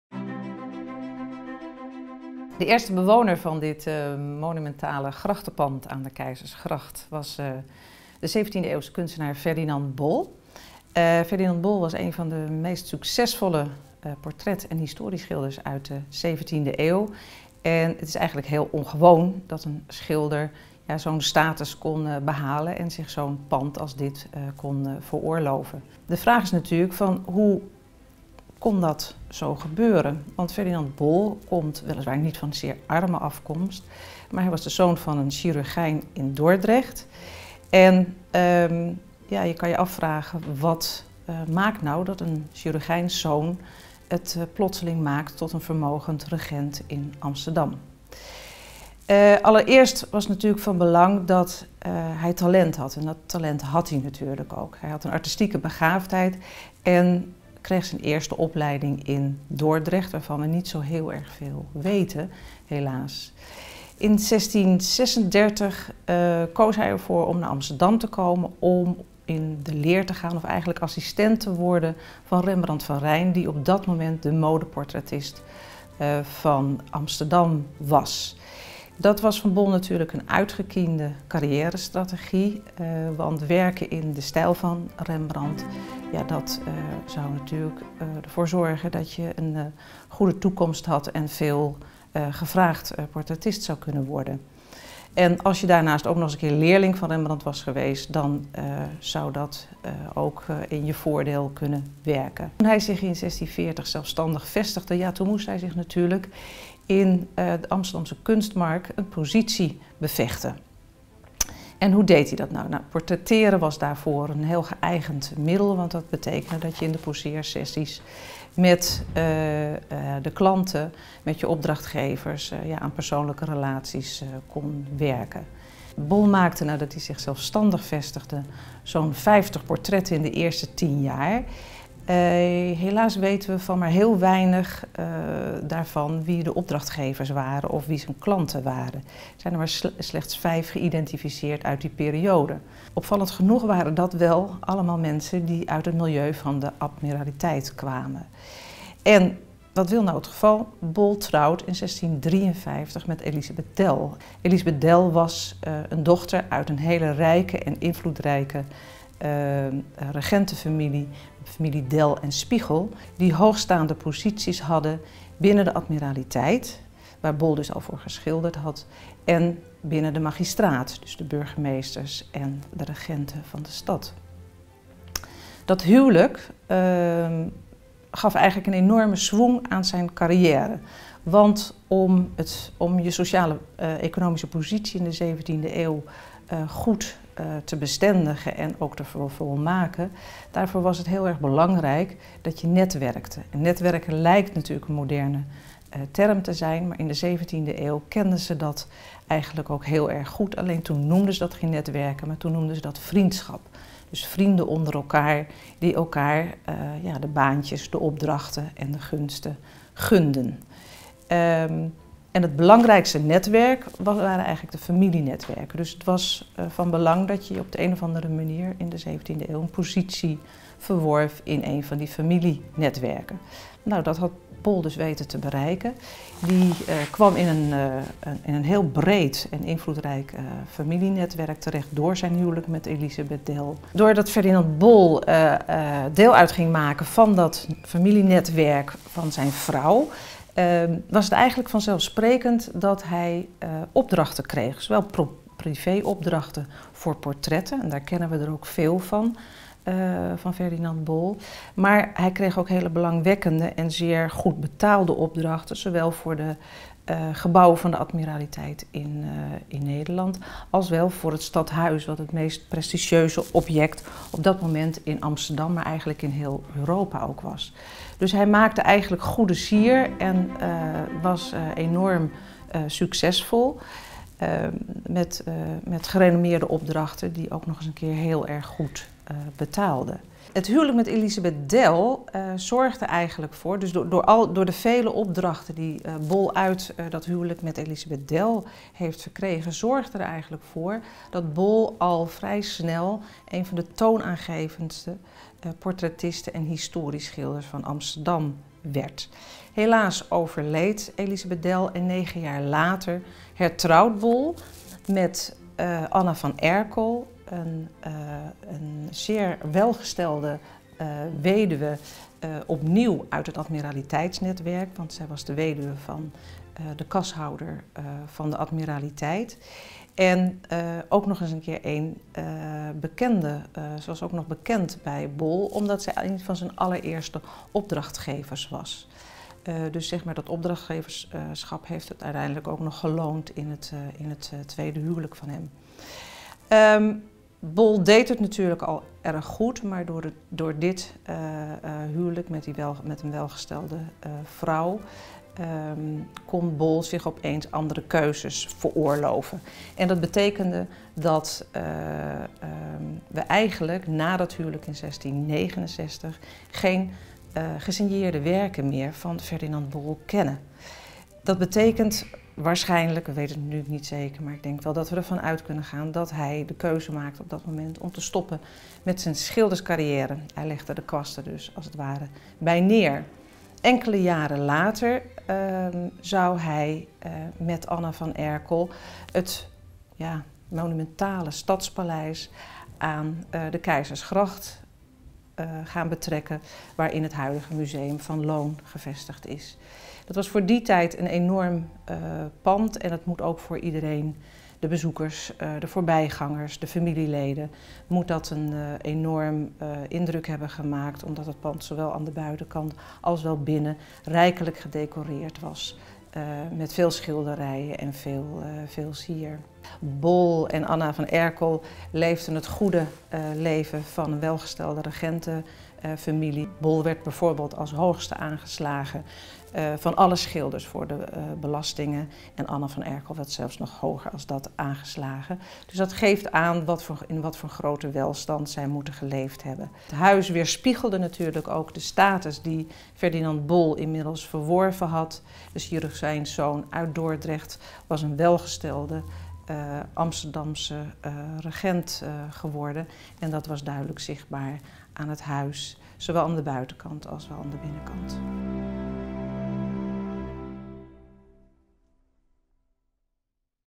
Ferdinand Bol: het huis, de collectie, de kunstenaar
Kunsthistoricus Erna Kok vertelt over Ferdinand Bol: wat verzamelde hij en hoe kon een schilder zich een pand aan één van de duurste Amsterdamse grachten veroorloven?